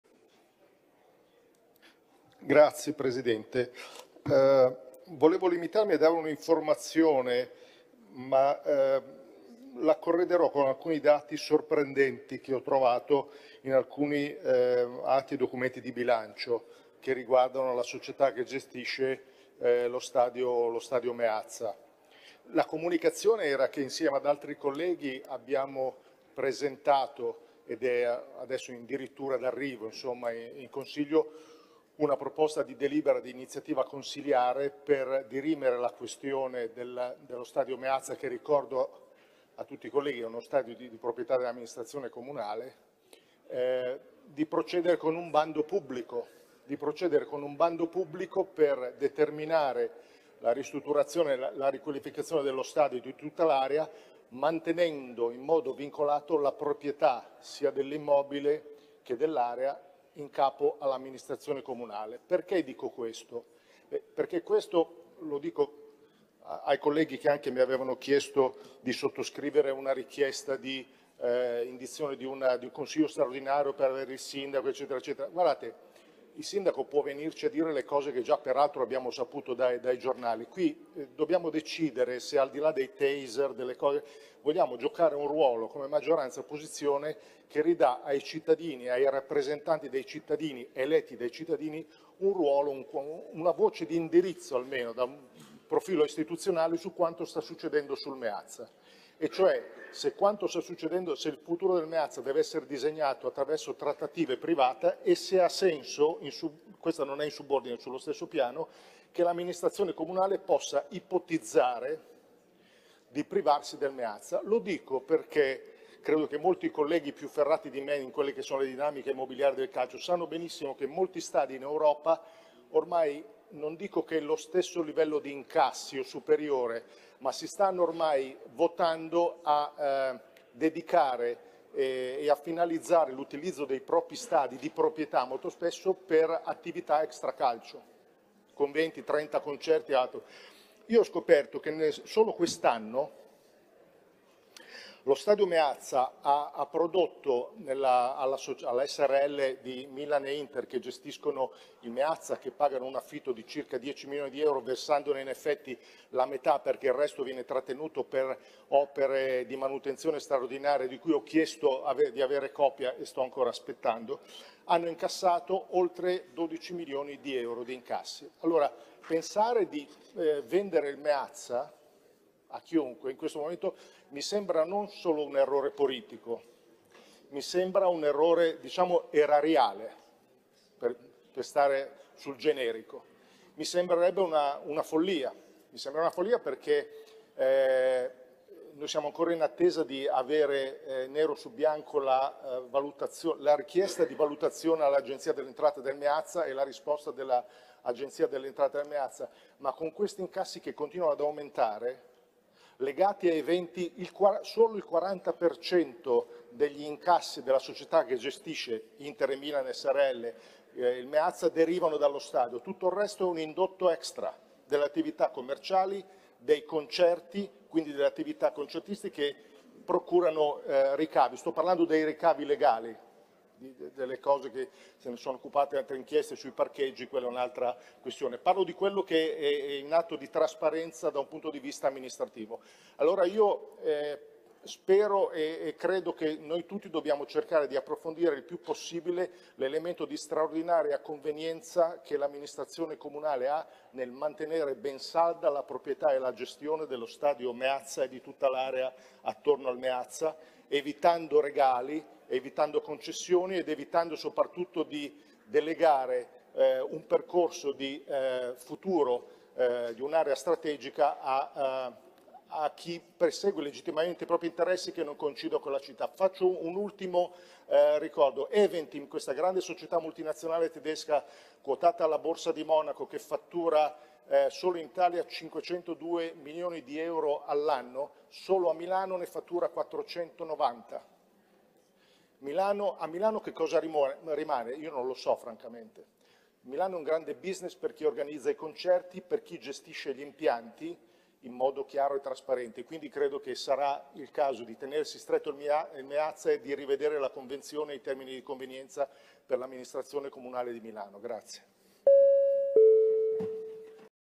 Intervento Enrico Fedrighini Consiglio 04 Novembre 2024
Intervento Enrico Fedrighini - Gruppo Misto Consiglio Milano del 04 Novembre 2024